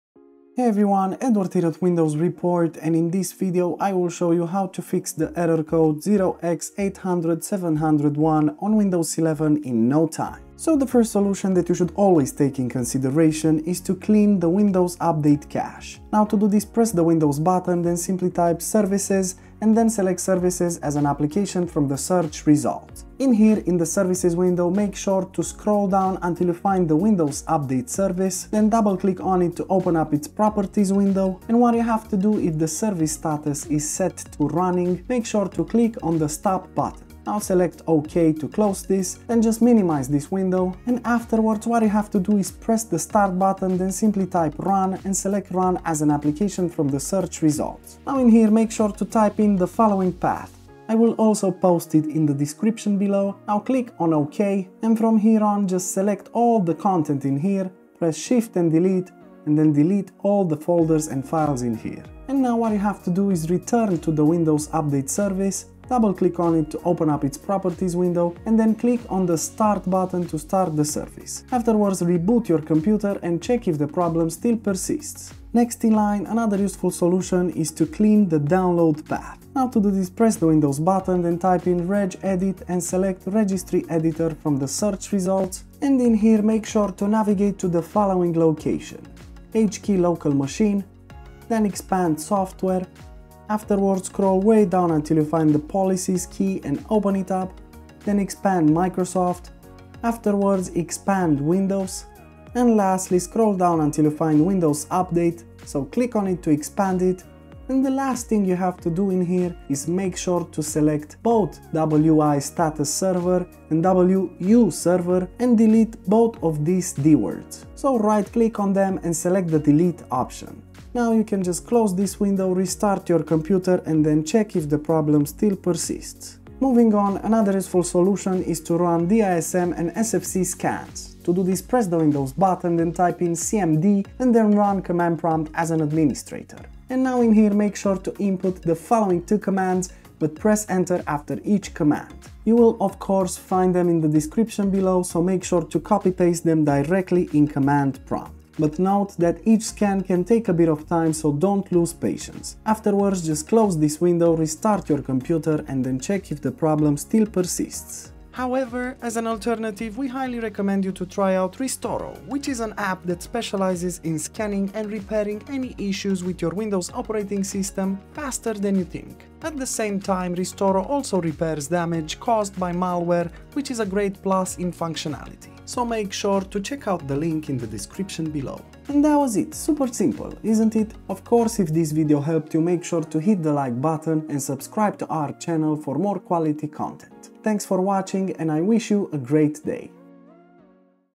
How to Fix the 0x8007001 Error Code on Windows 11
| ⚙️Try Fortect to fix and optimize your PC like a Pro

Take control of error 0x8007001 with our comprehensive guide. Learn to fix the most common issues and enjoy a frustration-free updating experience. Keep your PC protected and up-to-date with ease! 💻🛠️

Run path:
%windir%\SoftwareDistribution\DataStore

Commandlines:
1. sfc /scannow
2. Dism /Online /Cleanup-Image /RestoreHealth

0:00 - Intro
0:12 - Clean the Windows Update Cache
1:15 - Clean the Download Path
2:07 - Run DISM and SFC Scans
2:38 - Restoro